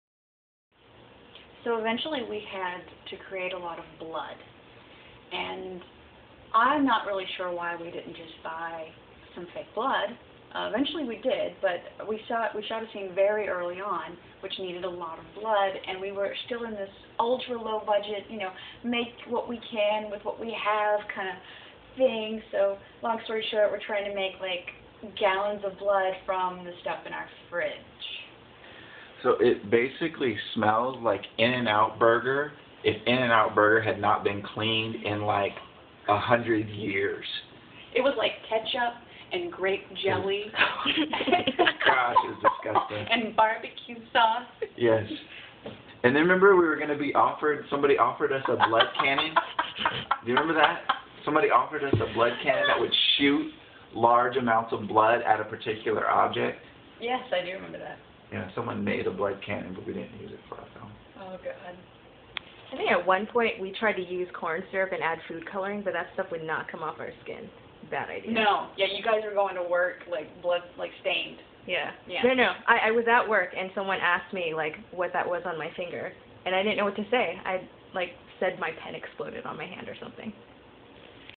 Why Does This Blood Taste So Good?
The trio from "Violent Night: The Movie" talk about blood... lots and lots of stinky, edible blood.